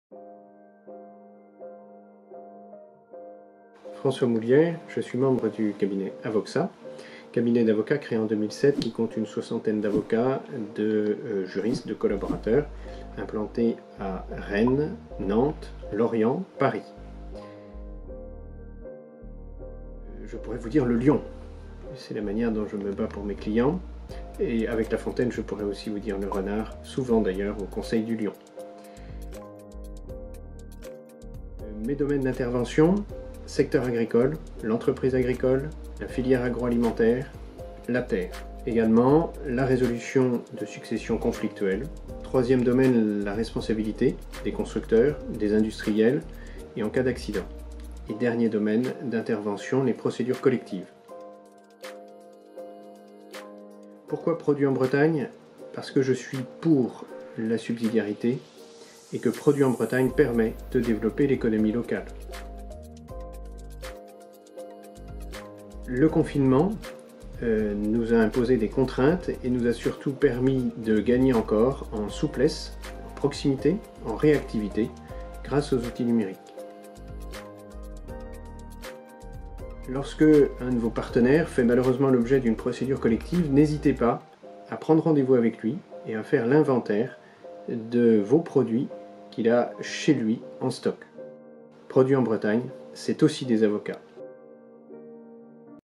Produit en Bretagne, c'est aussi... des avocats
François Moulière, Avocat associé au sein du cabinet Avoxa, nous explique pourquoi ils sont membres du Réseau Produit en Bretagne.
Plus de 50% des membres du Réseau Produit en Bretagne sont des entreprises de Services, d'Ingénierie ou de la Culture.